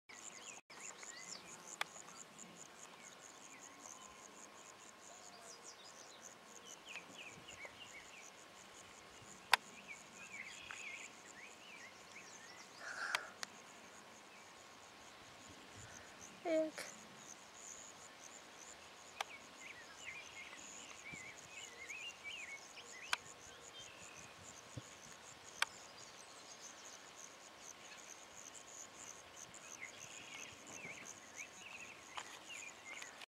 Snake at Memphis Lake, Memphis Nebraska. 2012
This snake is not native to Nebraska.